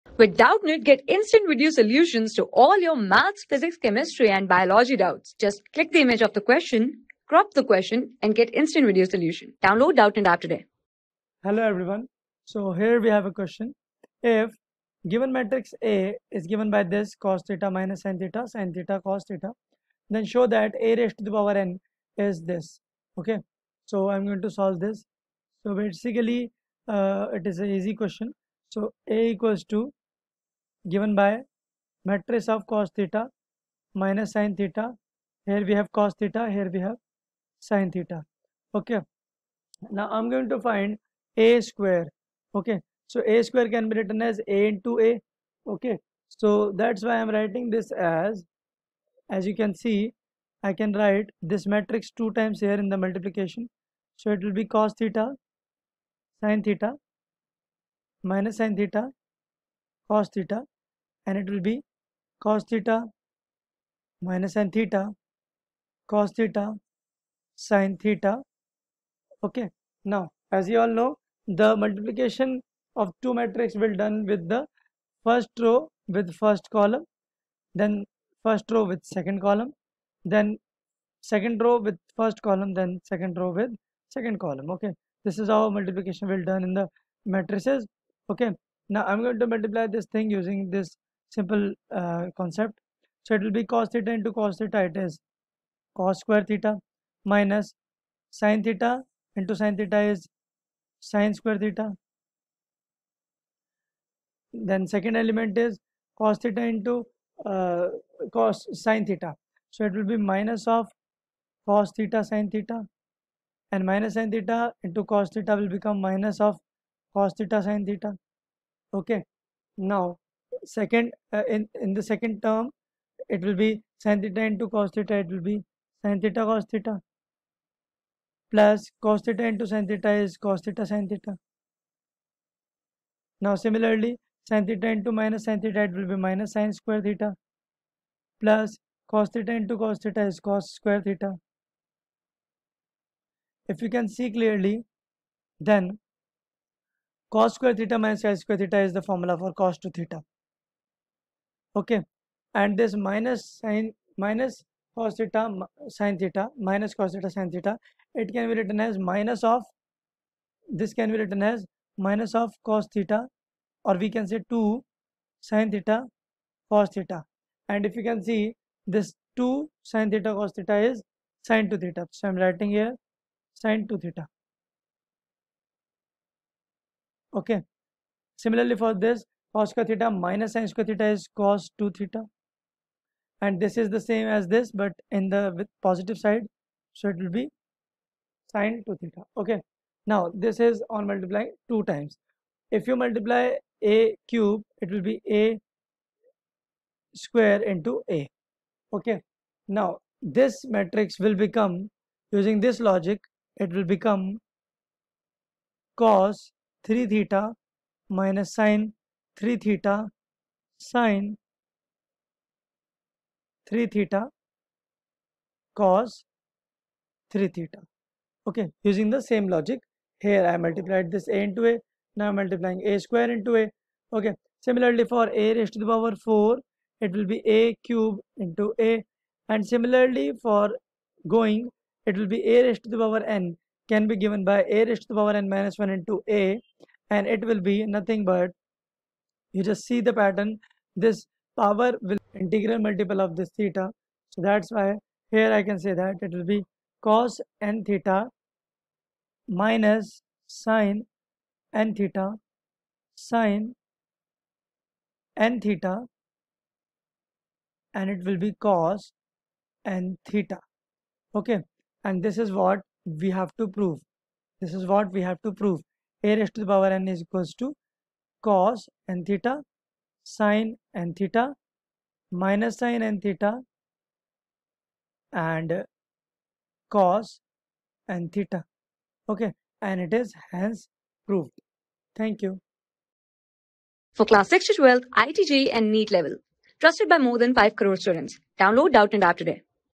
If A=[[costheta,-sintheta],[sintheta,costheta]] then show that A^n = [[cosntheta,-sinntheta],[si...
If A=[[costheta,-sintheta],[sintheta,costheta]] then show that A^n = [[cosntheta,-sinntheta],[sinntheta,cosntheta]]
Class: 12
Subject: MATHS
Chapter: MATRICES - FOR BOARDS
Board:IIT JEE

👉 Have Any Query? Ask Us.